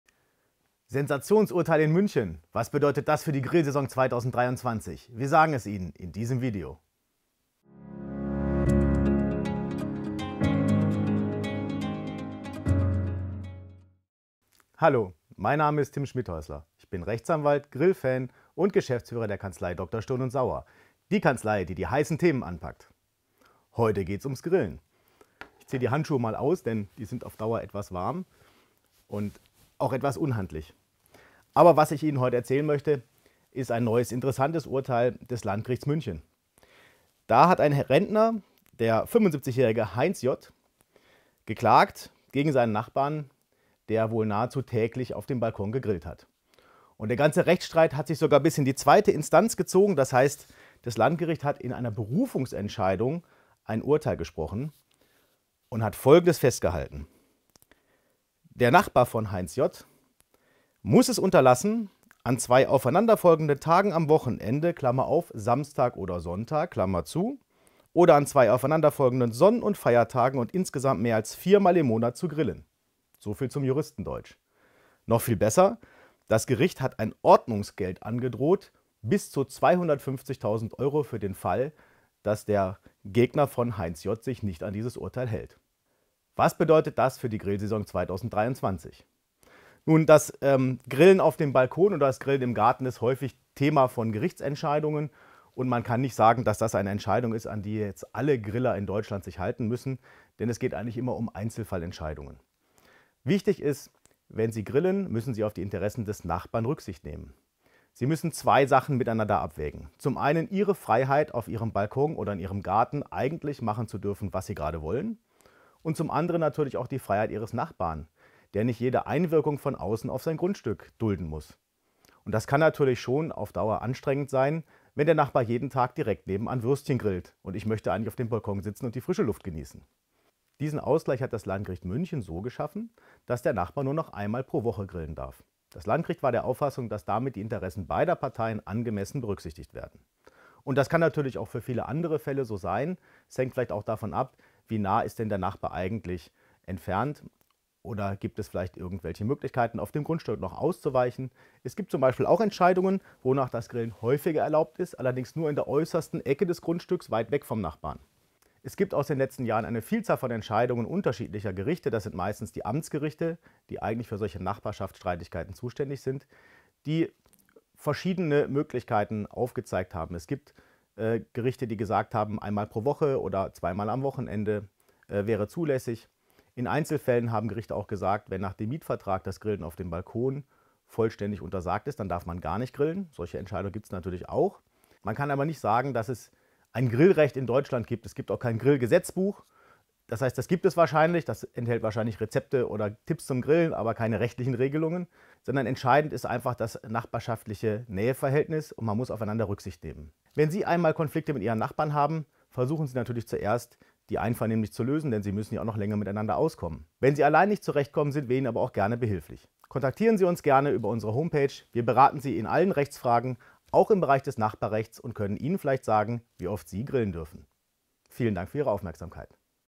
Wenn der Nachbar im Grillrauch steht
Mit dem Beginn des Frühlings starten viele von uns in die Grillsaison. Wie oft darf ich überhaupt in der Woche grillen, ohne meine Nachbarn zu stören? Anwalt und Geschäftsführer Tim Schmidhäußler erklärt die nachbarschaftlichen Fallstricke beim Grillen.

▬ Social Media ▬